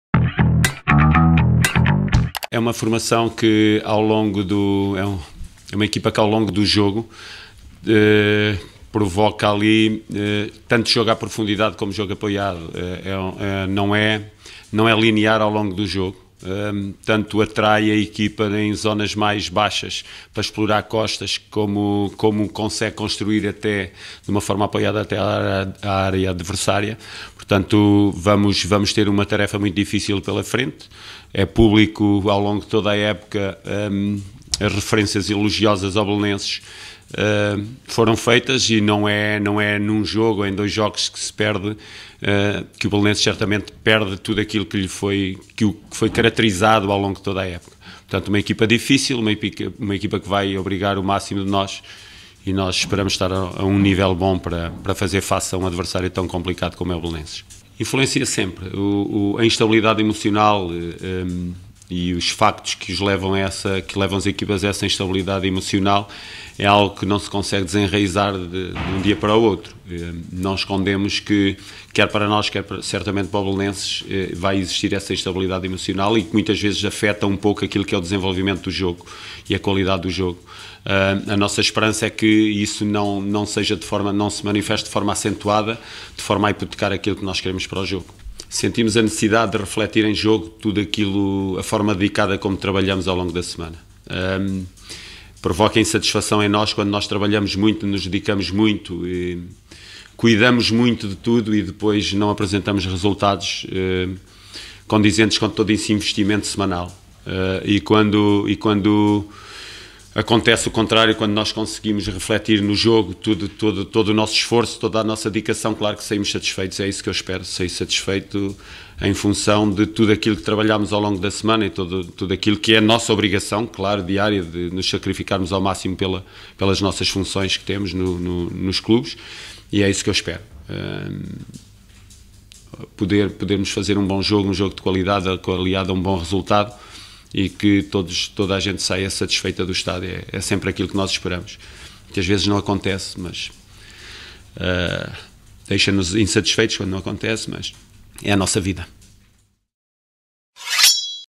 Luís Castro: “Espero que toda a gente saia satisfeita“
Luís Castro faz antevisão ao encontro com o Belenenses SAD, encontro referente à 33ª e penúltima jornada da Liga NOS 2018/19.